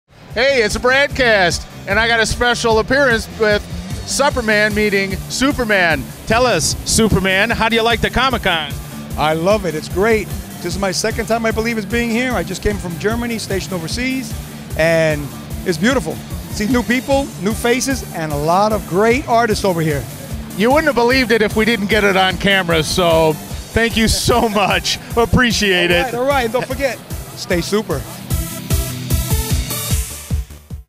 Supperman v Superman | East Coast Comicon 2017 | Comics, Beer & Sci-fi
Brad Levin (aka The Bradcast, aka Supperman) talks to Superman cosplayer at the 2017 East Coast Comic Con.